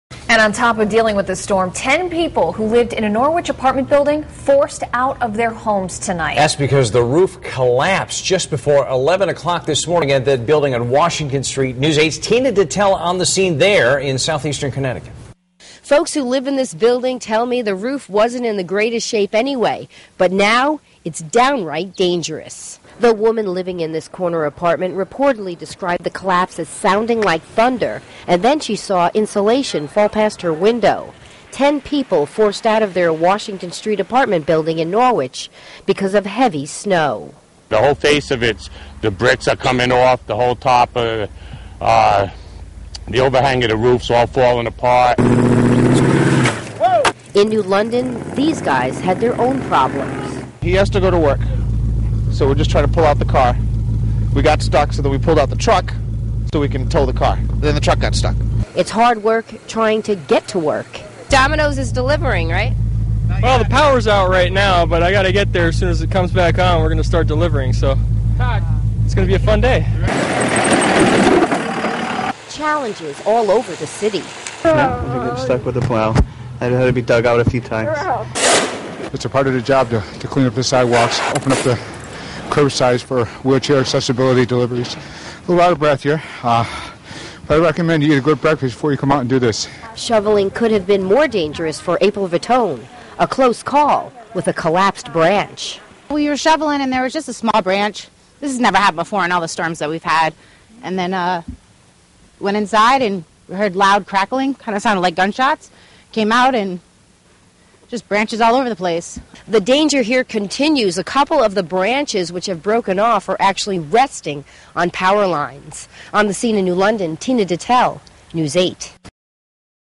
Roof collapse at Norwich Apartment Bldg
Ten people have been forced out of their homes after a roof collapse at a Norwich apartment building Wednesday.